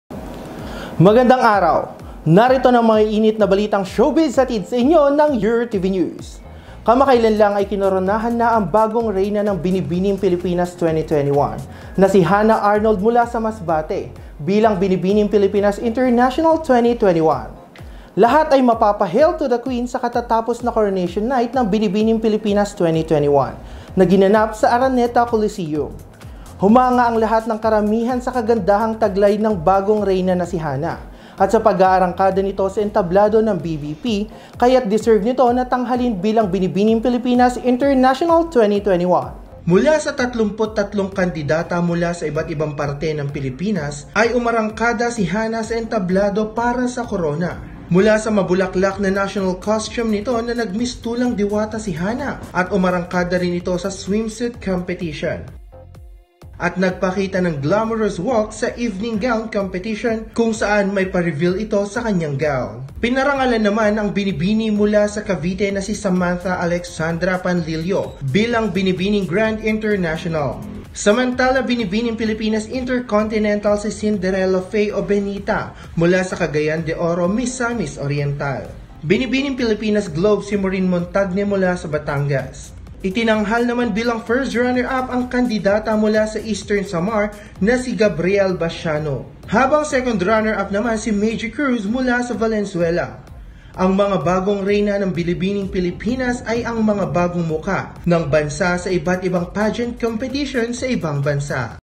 KINORONAHAN NA SI HANNAH ARNOLD MULA SA MASBATE BILANG BINIBINING PILIPINAS INTERNATIONAL SA
KINORONAHAN NA SI HANNAH ARNOLD MULA SA MASBATE BILANG BINIBINING PILIPINAS INTERNATIONAL SA BINIBINING PILIPINAS 2021